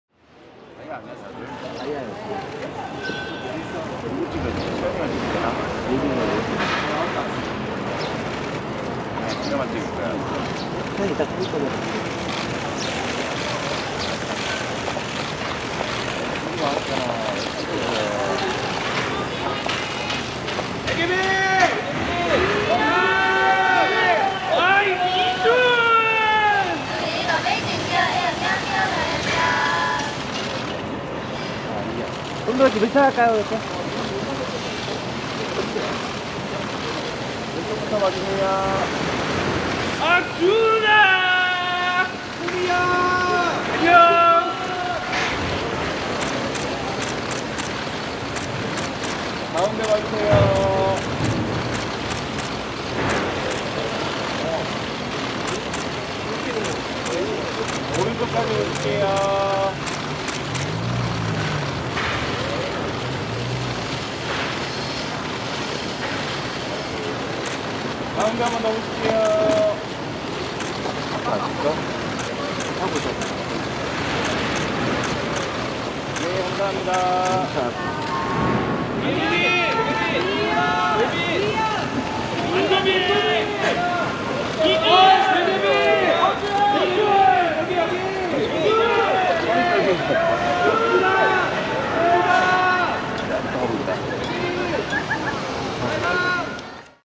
다이아 (DIA) @ 190412 뮤직뱅크 출근길
4월 12일 KBS 뮤직뱅크에 다이아가 출근하고 있다.